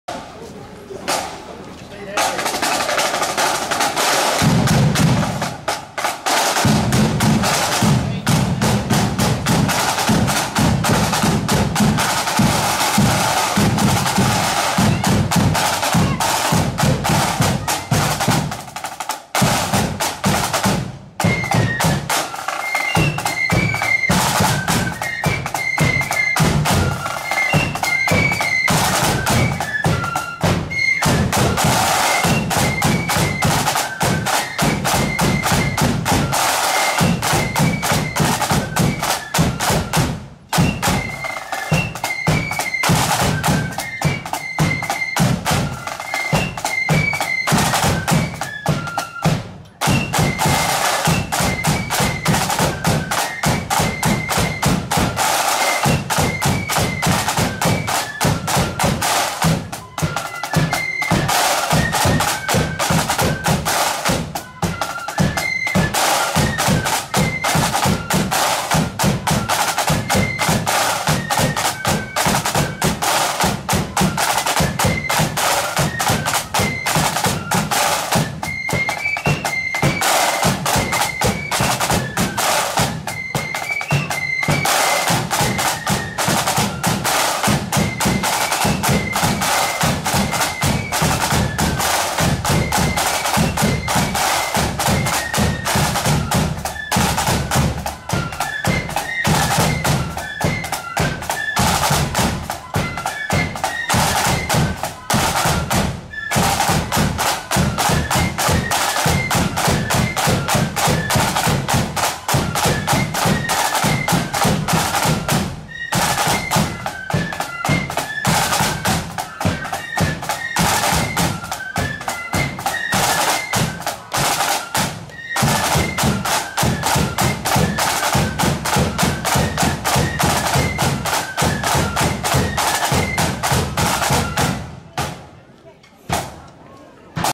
Drumaheagles Young Defenders (No.7) @ Their Own Parade 15/08/2020 (4K)
Drumaheagles Young Defenders Flute Band parading through Ballymoney on Saturday 15/08/2020. This was the date of their annual parade but due to the Covid restrictions they couldnt hold their usual parade with invited bands & instead paraded the Co. Antrim town on their own.